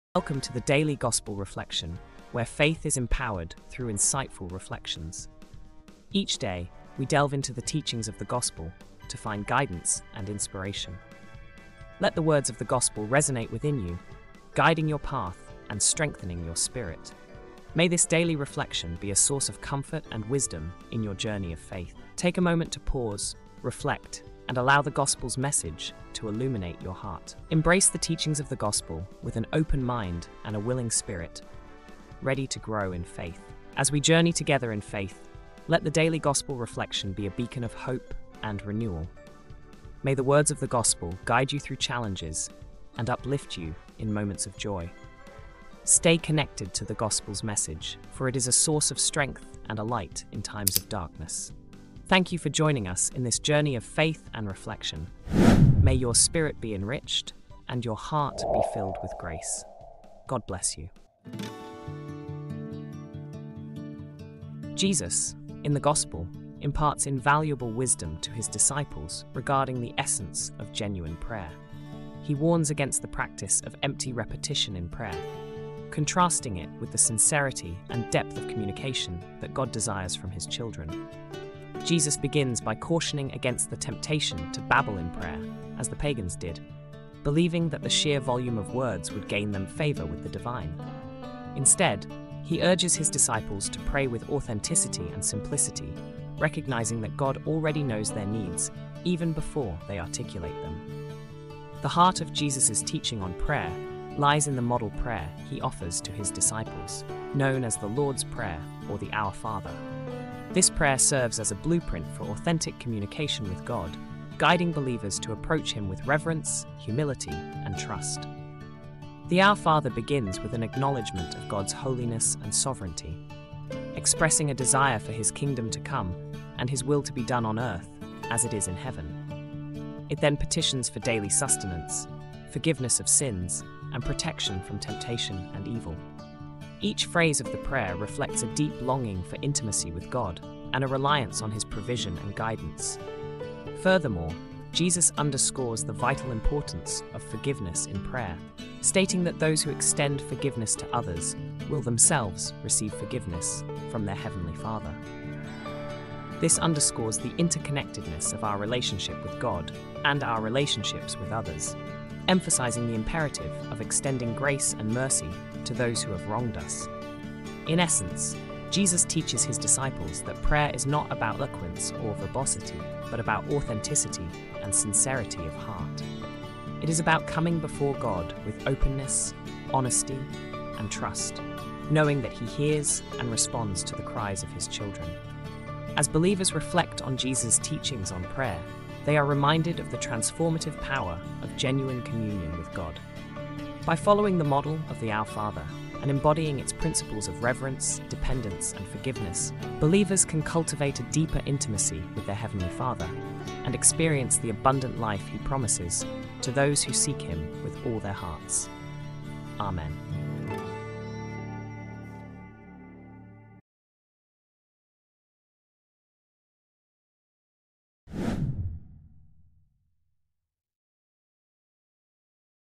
The Power of Authentic Prayer
Reflection
Jesus, in the Gospel, imparts invaluable wisdom to His disciples regarding the essence of genuine prayer. He warns against the practice of empty repetition in prayer, contrasting it with the sincerity and depth of communication that God desires from His children.

Jesus begins by cautioning against the temptation to babble in prayer, as the pagans did, believing that the sheer volume of words would gain them favor with the divine. Instead, He urges His disciples to pray with authenticity and simplicity, recognizing that God already knows their needs even before they articulate them.

The heart of Jesus’ teaching on prayer lies in the model prayer He offers to His disciples, known as the Lord’s Prayer or the Our Father. This prayer serves as a blueprint for authentic communication with God, guiding believers to approach Him with reverence, humility, and trust.

The Our Father begins with an acknowledgment of God’s holiness and sovereignty, expressing a desire for His kingdom to come and His will to be done on earth as it is in heaven. It then petitions for daily sustenance, forgiveness of sins, and protection from temptation and evil. Each phrase of the prayer reflects a deep longing for intimacy with God and a reliance on His provision and guidance.

Furthermore, Jesus underscores the vital importance of forgiveness in prayer, stating that those who extend forgiveness to others will themselves receive forgiveness from their Heavenly Father. This underscores the interconnectedness of our relationship with God and our relationships with others, emphasizing the imperative of extending grace and mercy to those who have wronged us.

In essence, Jesus teaches His disciples that prayer is not about eloquence or verbosity but about authenticity and sincerity of heart. It is about coming before God with openness, honesty, and trust, knowing that He hears and responds to the cries of His children.

As believers reflect on Jesus’ teachings on prayer, they are reminded of the transformative power of genuine communion with God. By following the model of the Our Father and embodying its principles of reverence, dependence, and forgiveness, believers can cultivate a deeper intimacy with their Heavenly Father and experience the abundant life He promises to those who seek Him with all their hearts. AMEN.